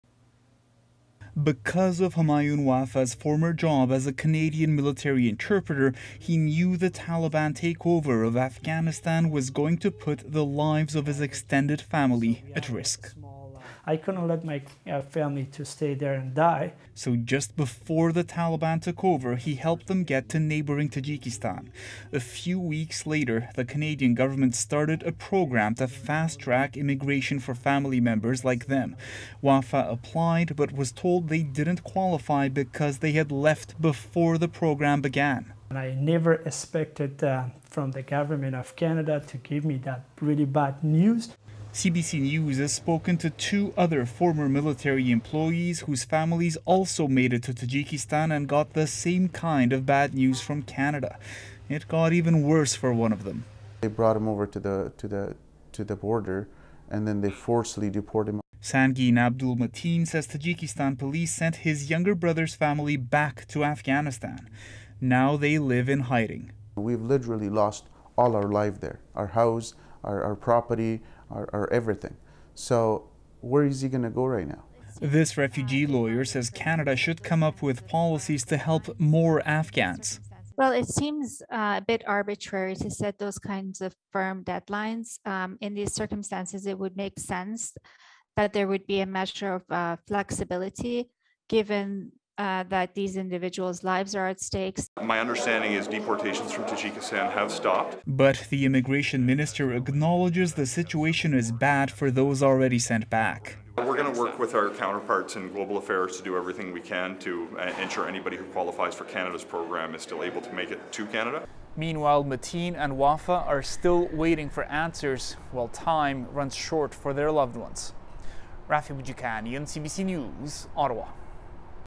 Afghans trying to come to Canada stuck in limbo
Three former Afghan employees of the Canadian military say their families got stuck in Tajikistan after being told by Ottawa they don't qualify for its resettlement program because they left Afghanistan before the Taliban takeover. Now, they worry for their family members’ safety.